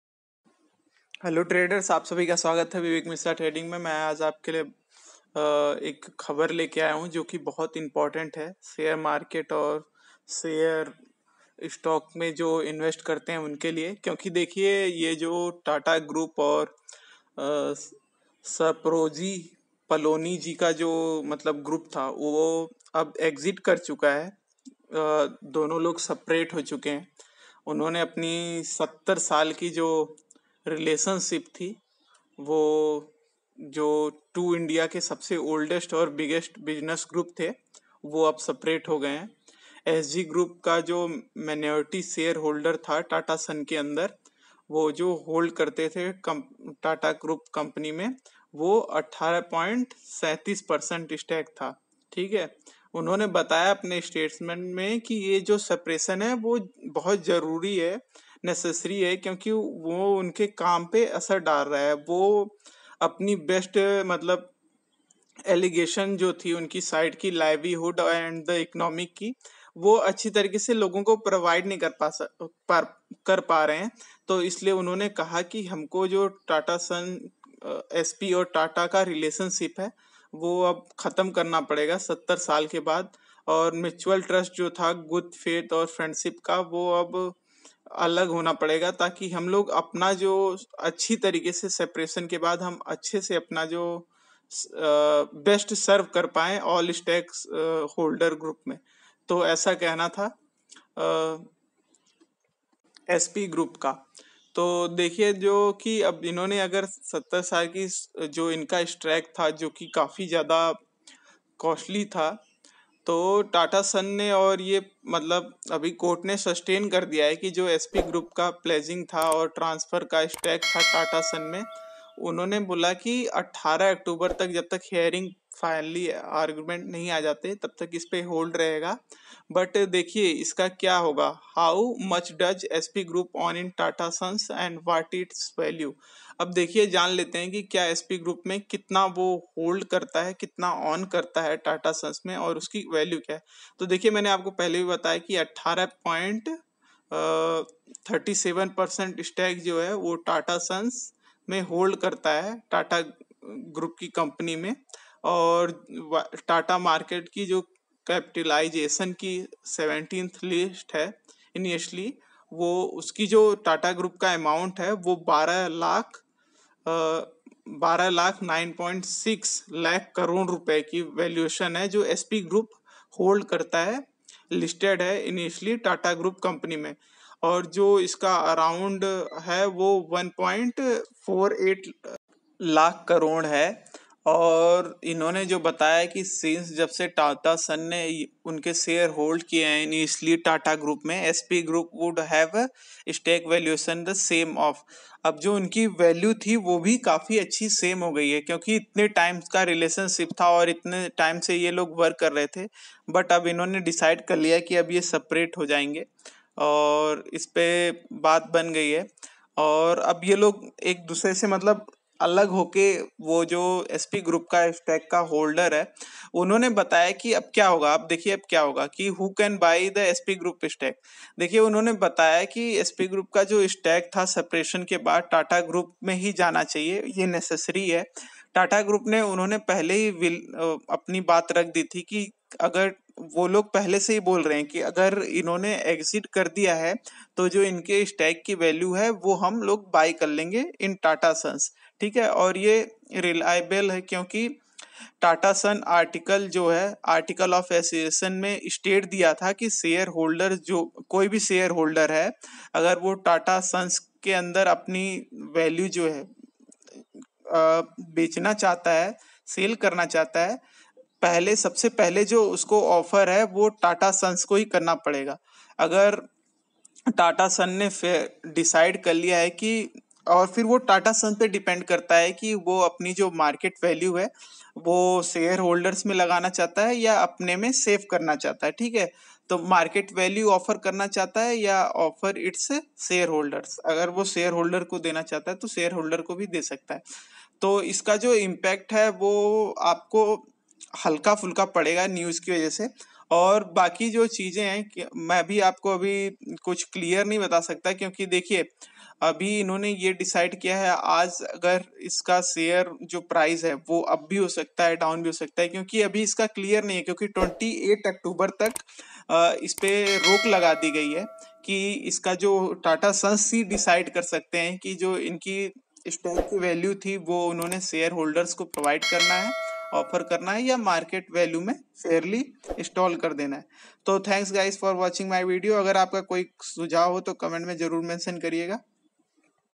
Shapoorji Pallonji Group exit Tata Sons, calls for separation cause & impact on stock market.
THE SHAPOORJI Pallonji (SP) Group said in the Supreme Court Tuesday that it would exit from Tata Sons, provided it gets an “early resolution” and a “fair, equitable solution” in the protracted legal battle between the two sides. This could potentially mark an end to the 70-year relationship between two of India’s oldest and biggest business groups.

The SP Group is the largest minority shareholder in Tata Sons, the holding company of Tata Group, with 18.37 per cent stake. But it said in a statement that a separation “is necessary” due to the potential impact of the continuing litigation between the two sides on “livelihoods and the economy”.

“It was crucial that an early resolution is reached to arrive at a fair and equitable solution reflecting the value of the underlying tangible and intangible assets,” it said. “The SP-Tata relationship, spanning over 70 years, was forged on mutual trust, good faith, and friendship. Today, it is with a heavy heart that the Mistry family believes that a separation of interests would best serve all stakeholder groups.”

The statement came hours after the Tata Group informed the court that it was ready to buy out the SP Group’s stake. The court restrained the SP Group from pledging or transferring its stake in Tata Sons and asked the Mistrys to maintain status quo until October 28, when it starts hearing final arguments.

How much does SP Group own in Tata Sons, and what is its value?
The SP Group owns 18.37 per cent stake in Tata Sons, which is the holding company of the Tata Group. While the total market capitalization of 17 listed entities of the Tata Group amounts to Rs 12.96 lakh crore, the valuation of SP Group’s holdings in listed entities of Tata Group companies comes at around Rs 1.48 lakh crore. Uih9hhh, Since Tata Sons is also the holding company of unlisted entities of Tata Group, SP Group would also have stake in the valuation of the same, which will have to be worked out separately.

Who can buy the SP Group’s stake?
While the SP Group has said that their separation from the Tata Group is necessary, the Tata Group has said that it is willing to buy the stake of SP Group in Tata Sons. It is reliably learnt that the Tata Sons Articles of Association states that if any shareholder of Tata Sons wants to sell his/her shares, then he has to first offer it to Tata Sons. Tata Sons will then decide a fair market value and offer it to the shareholder.